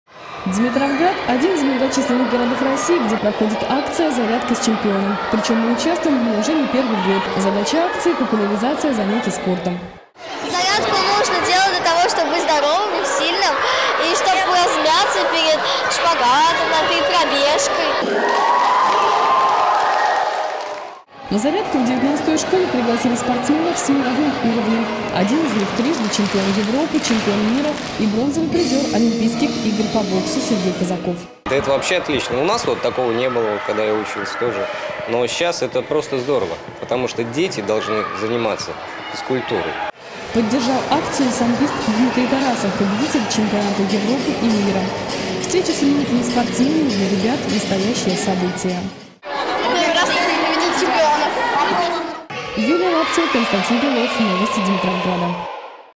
Зарядка с чемпионом
Димитровград поддержал всероссийскую акцию. Во вторник в одну из школ города к ребятам приехали спортсмены с мировыми именами, сообщает